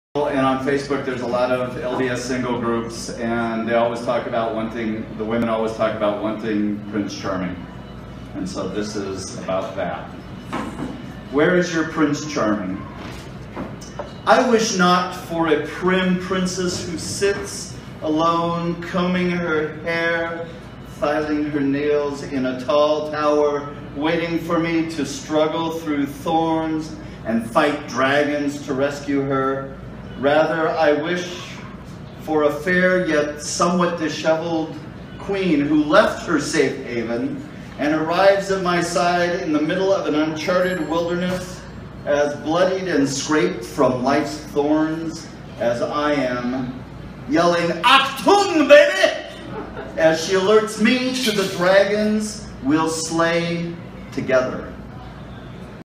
YourPrinceCharming SFYS YouTube May14 2015
Performance Of "Where is Your Prince Charming?" (seen on my blog ) -- at Speak for Yourself poetry open mic at Enliten Cafe, Provo, Utah, May 14, 2015.